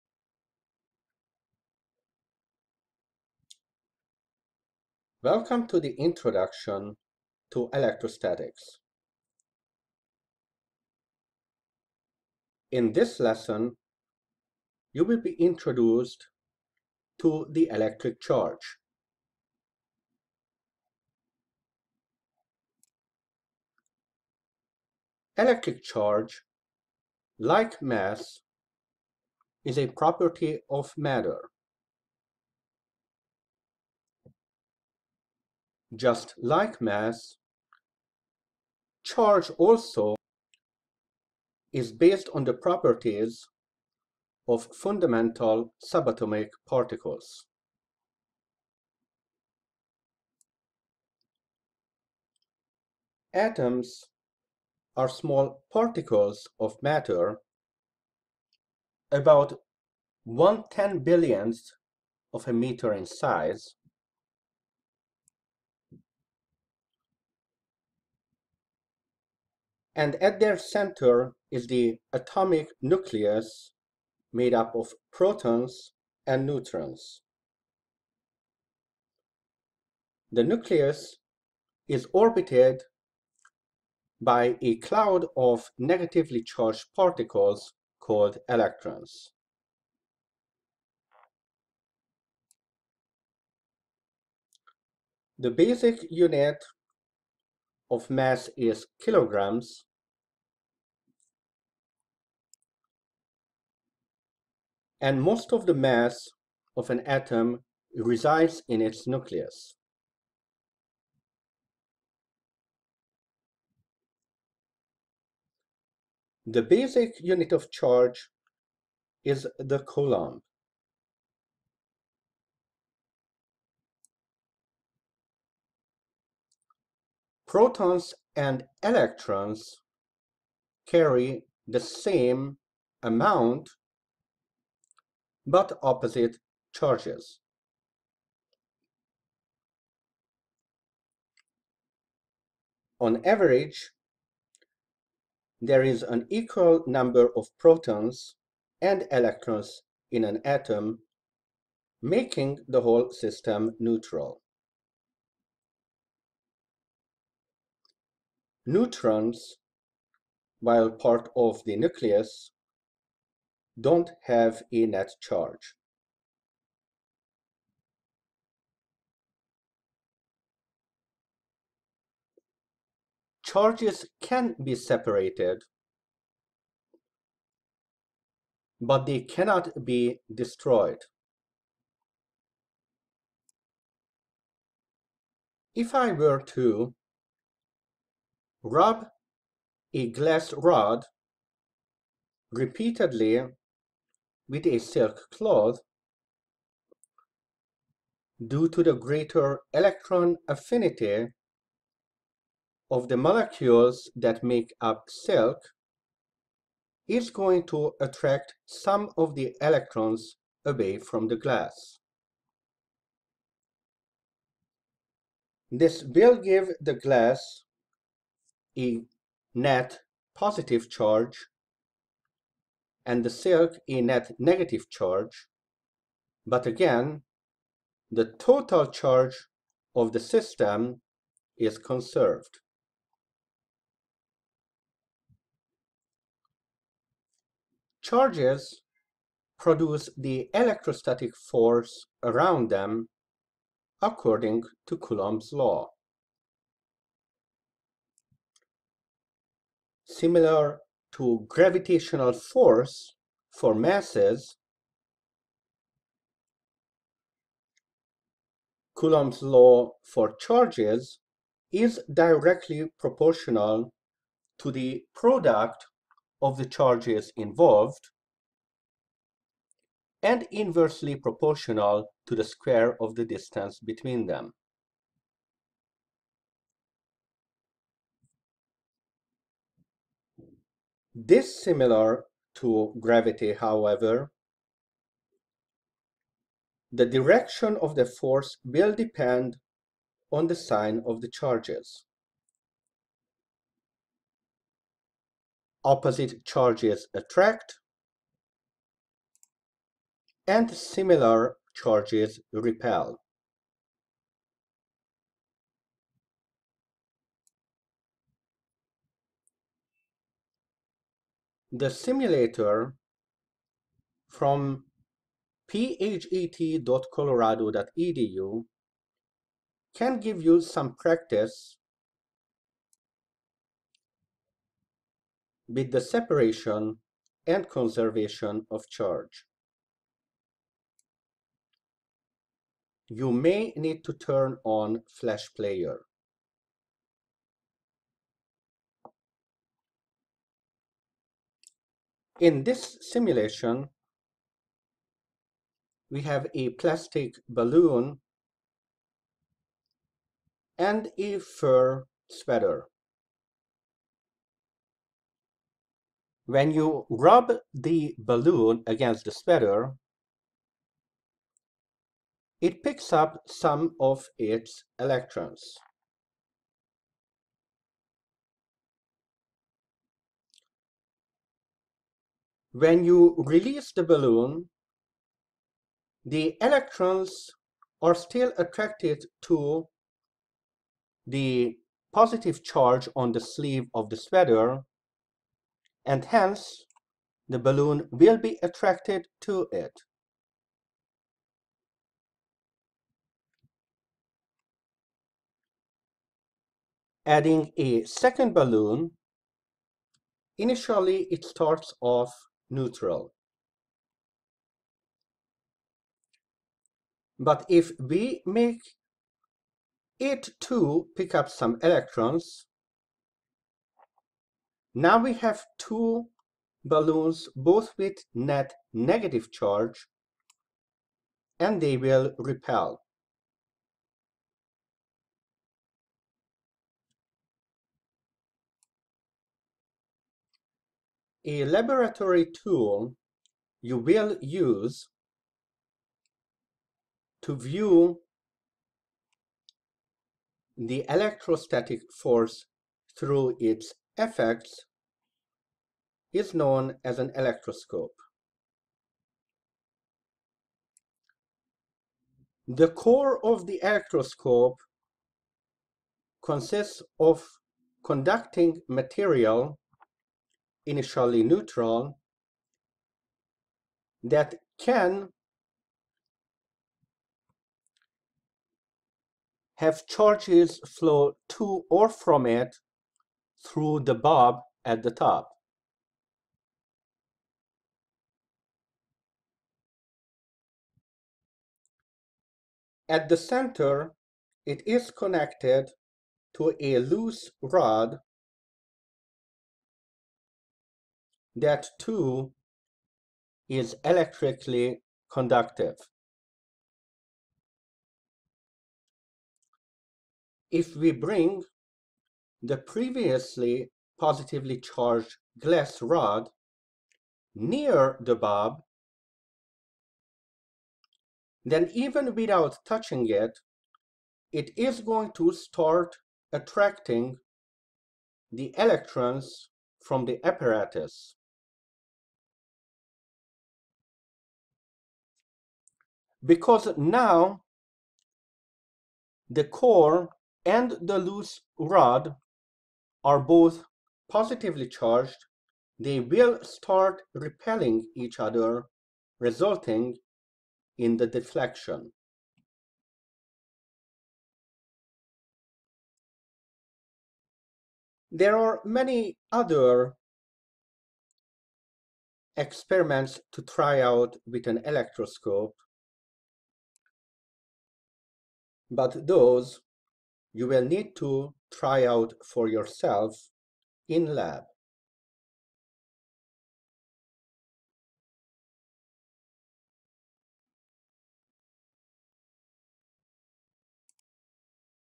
Introduction to Electrostatics: the Electric Charge
Atomic theory/basics of charge 0:53
Coulomb's law 3:57
Balloon-sweater demonstration (simulator from phet.colorado.edu) 5:10
Electroscope 7:11